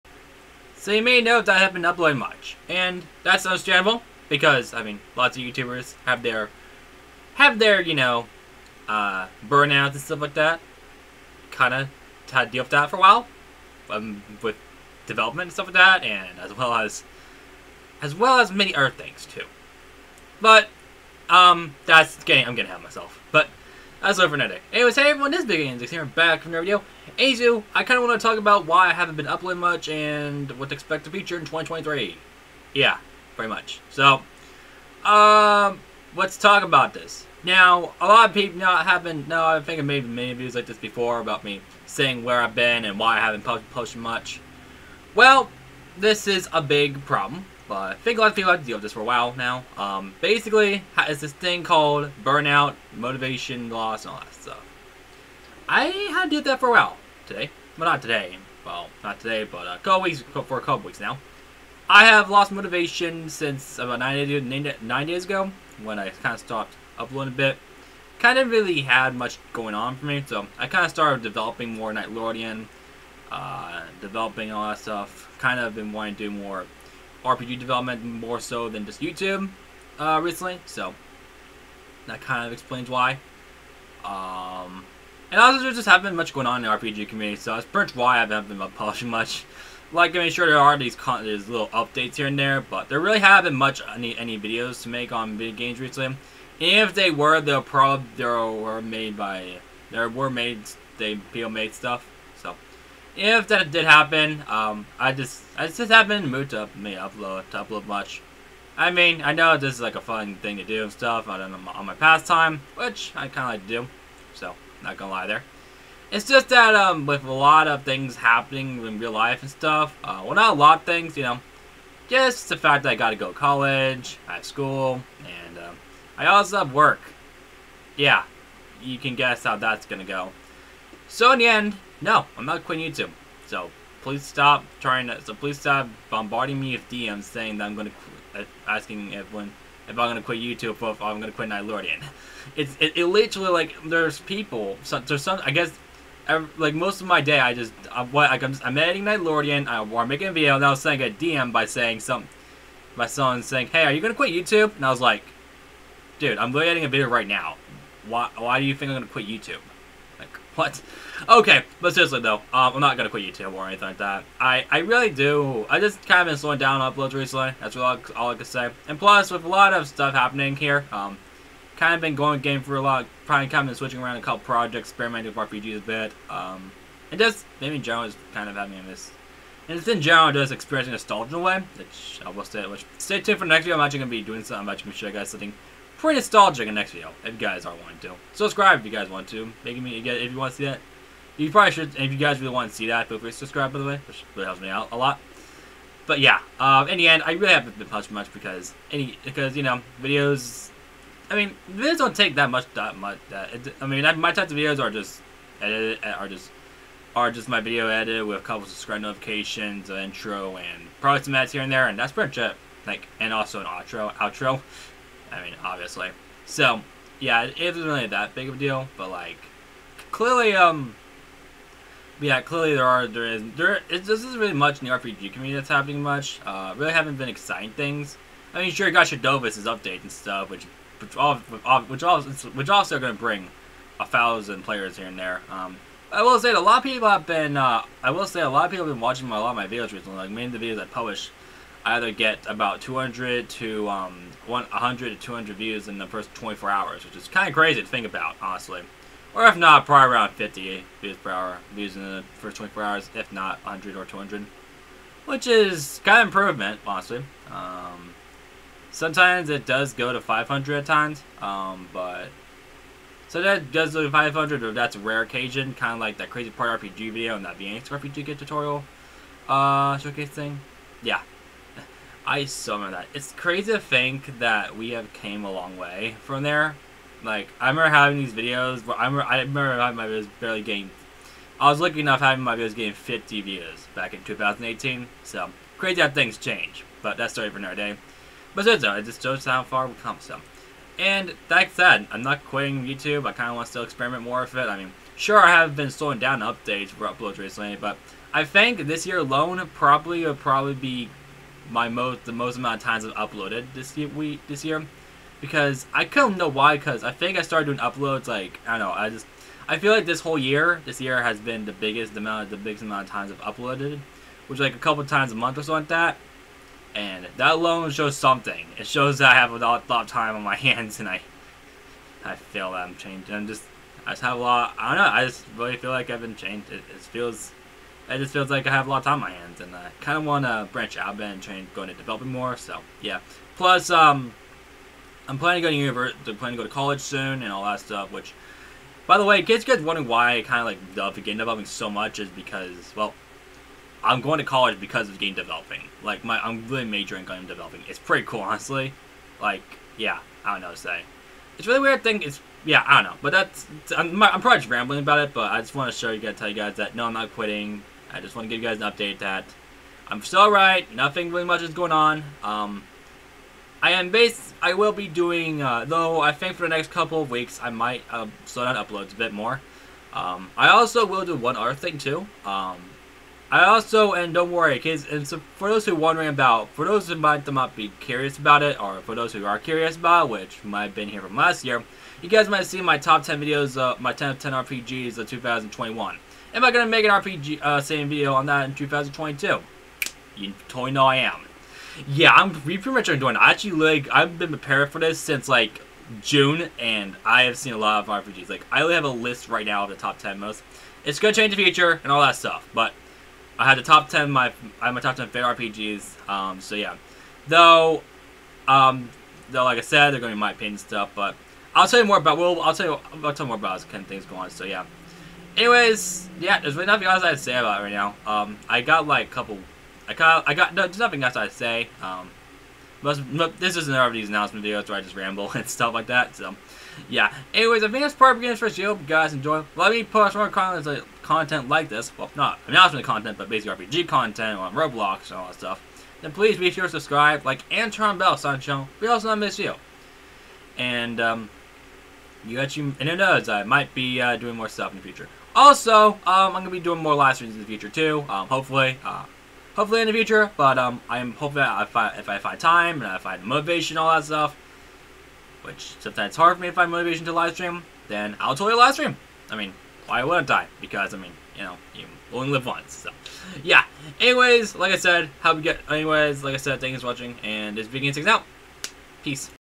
Quitting YouTube in 2023? (No, I'm just bored lol...) (BWKing16)
This video will go over where I have been and what to expect in 2023... No I am not quitting just been really busy with developing like usual. I have also been doing some stuff on my past projects, more about that later though...